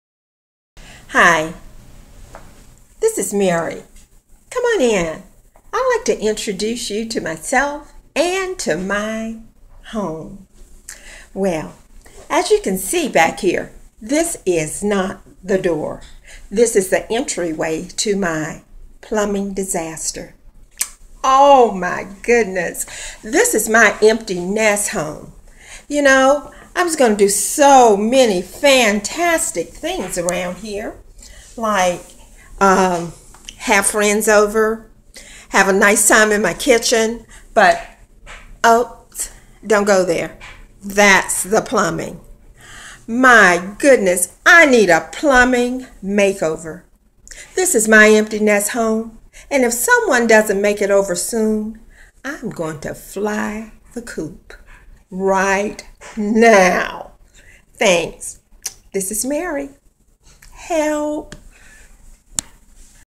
Empty Nester - Hole in Wall Disaster
This is no laughing matter. My moms home is crumbling on her daily...

She purchased her townhome about 8 or 9 years ago as a place to escape.  But for the past four years she is patching up walls, covering her had from a fallen ceiling.  In fact she dodged the water heater the first time which fell through her closet and damaged her wardrobe.

She is exhausted and truly doesn't have a clue what to do next.. Any extra money she has goes to plumbing which just informed her that she has faulty pipes.

We are afraid that this disaster could leave her homeless at any moment.  We need the help of Rescue my Renovation!